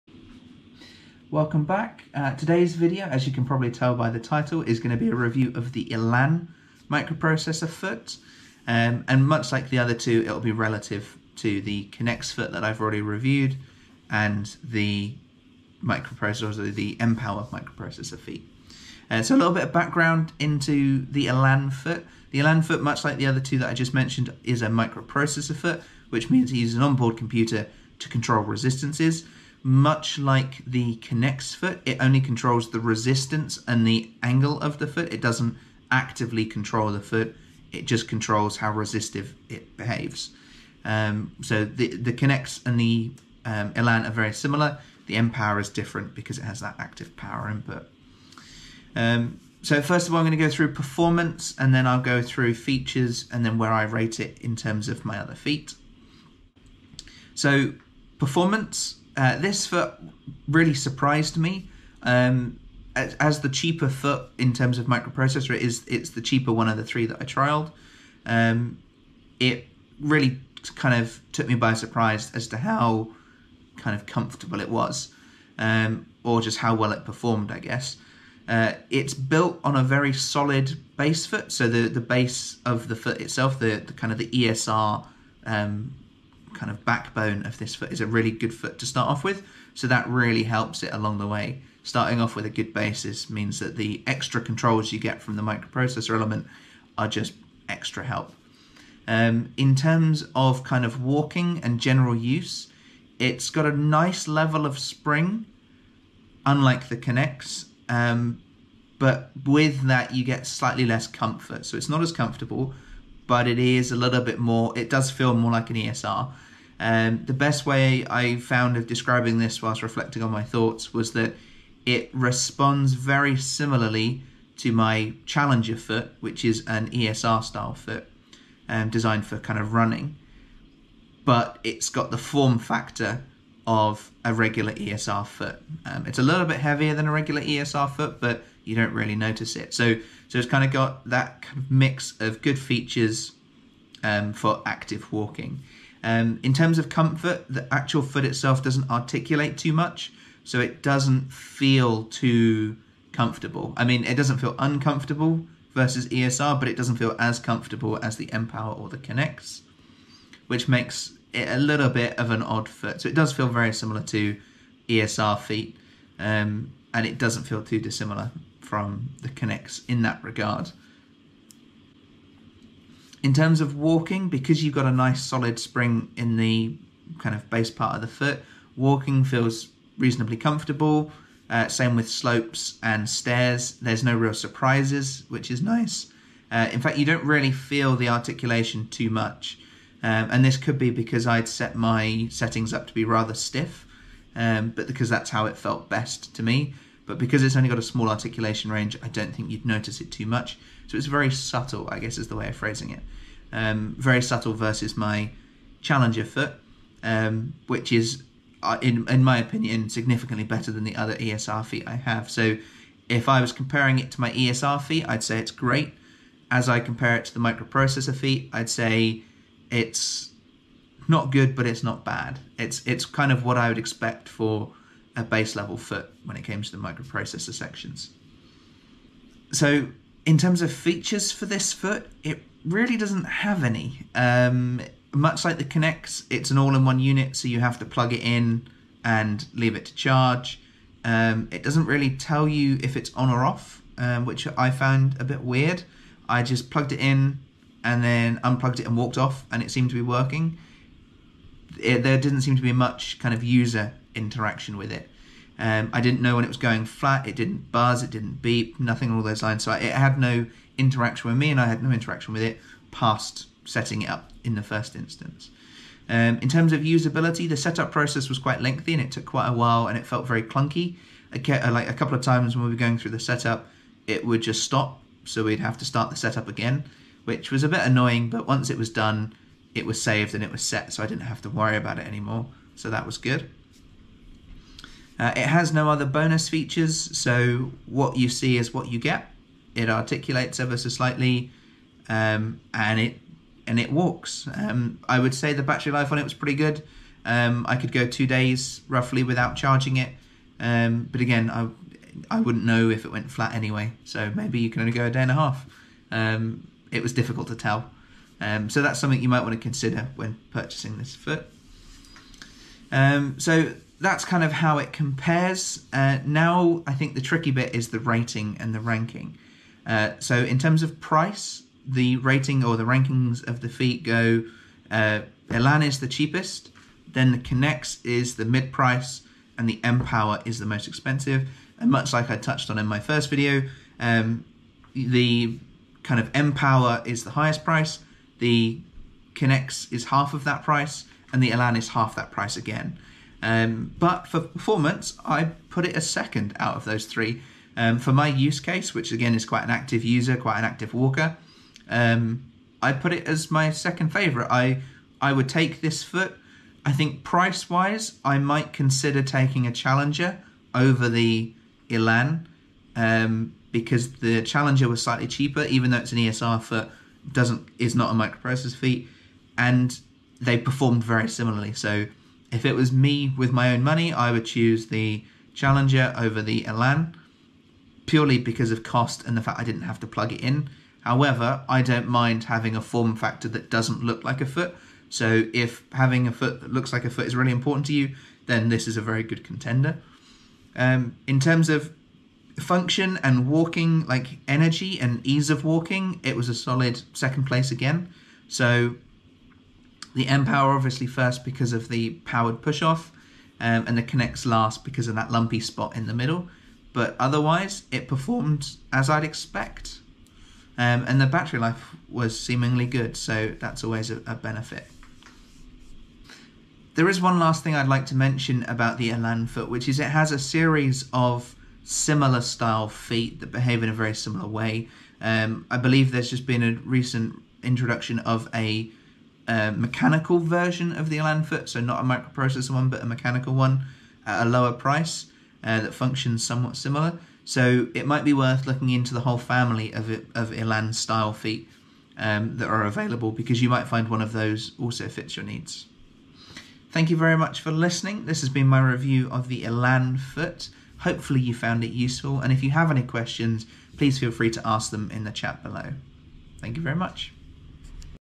Blatchford Elan Microprocessor Prosthetic Ankle Review
These are my thoughts of the Blatchford Elan after a two week trailing the foot as a left leg amputee. I compare this ankle to the other main microprocessor ankles available in the UK.